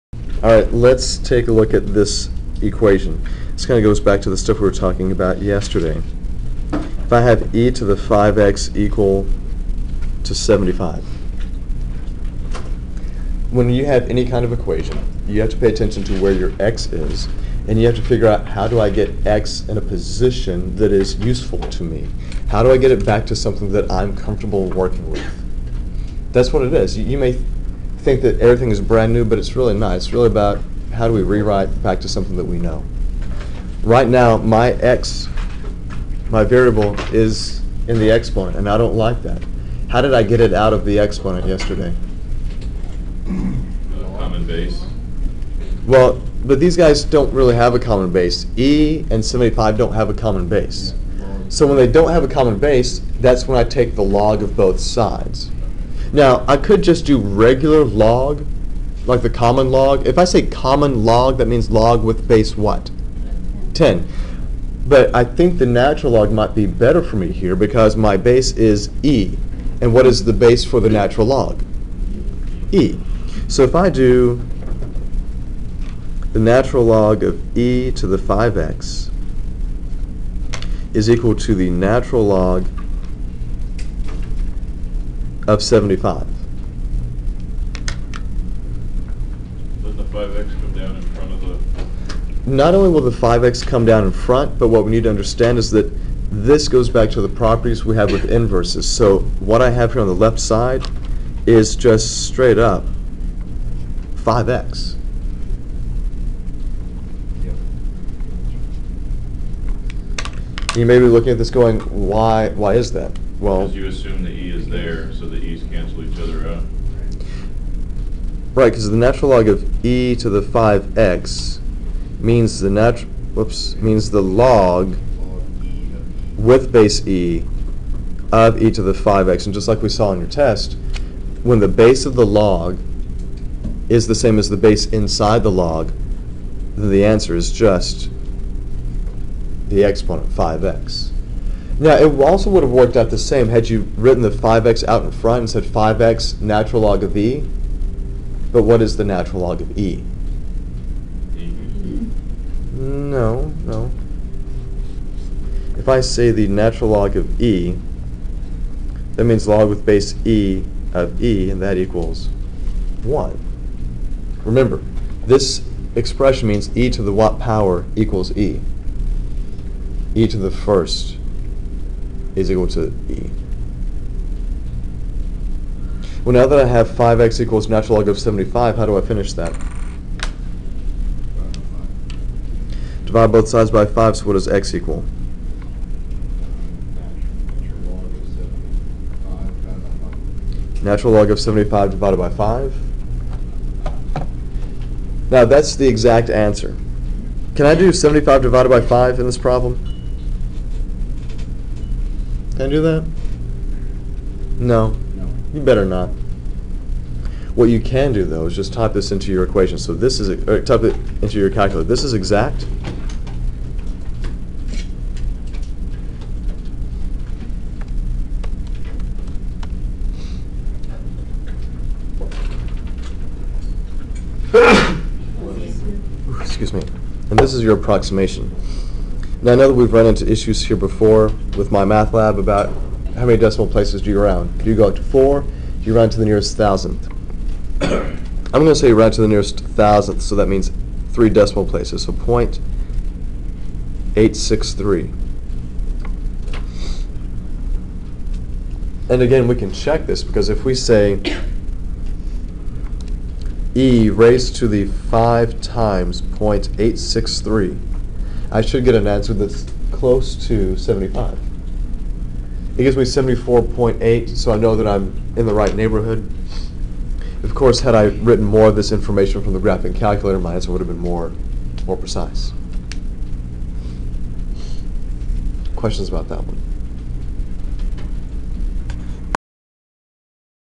College Algebra - Part 163 (Exponential Equations)
Solve:
e^(5x) = 75